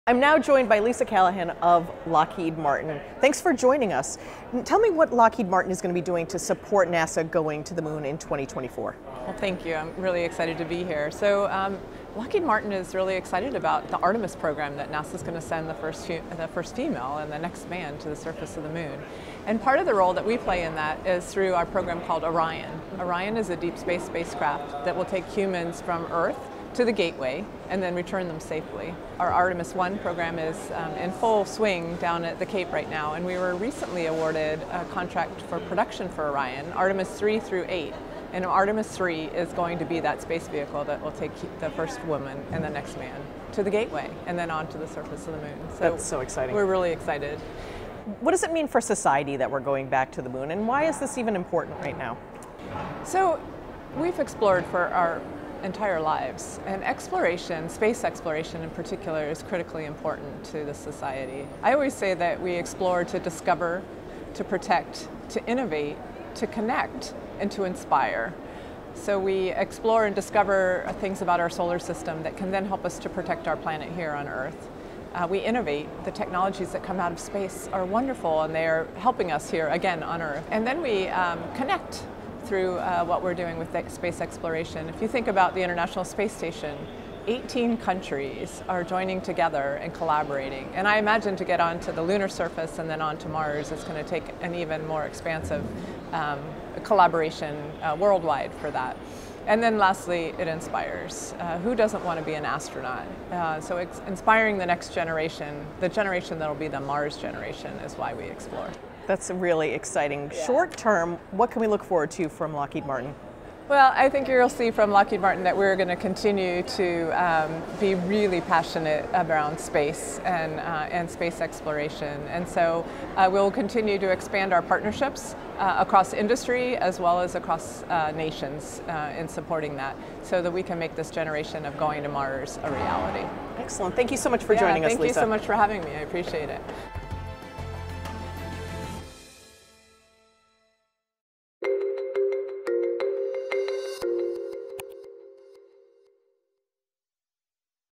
Interview with Lisa Callahan - IAC 2019
At the 70th International Astronautical Congress, in Washington, D.C., IAC TV sits down with Lisa Callahan, VP and GM of Commercial Civil Space at Lockheed Martin, to talk about how Lockheed Martin is supporting NASA in its mission to the moon. What this means for society at large that we’re going back to the moon and why it matters, and what more we can expect from the company.